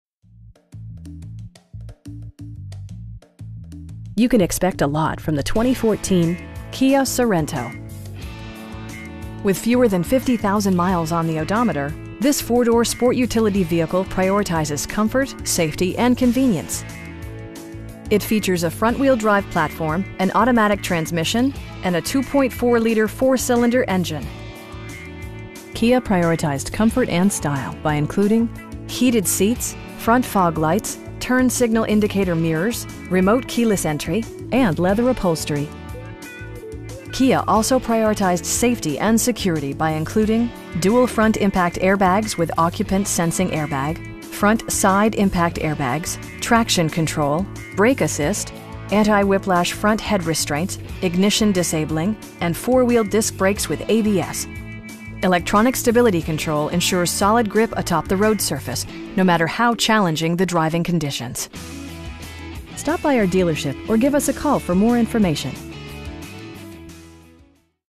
2014 Kia Sorento EX in Stamford, CT 06902
You can expect a lot from the 2014 Kia Sorento. With fewer than 50,000 miles on the odometer, this 4 door sport utility vehicle prioritizes comfort, safety and convenience. It features a front-wheel-drive platform, an automatic transmission, and a 2.4 liter 4 cylinder engine. Kia prioritized comfort and style by including: heated seats, front fog lights, tilt and telescoping steering wheel, turn signal indicator mirrors, power windows, remote keyless entry, and leather upholstery. Kia also prioritized safety and security by including: dual front impact airbags with occupant sensing airbag, front SIDE impact airbags, traction control, brake assist, anti-whiplash front head restraints, ignition disabling, and 4 wheel disc brakes with ABS. Electronic stability control ensures solid grip atop the road surface, no matter how challenging the driving conditions. We pride ourselves in consistently exceeding our customer's expectations. Stop by our dealership or give us a call for more information.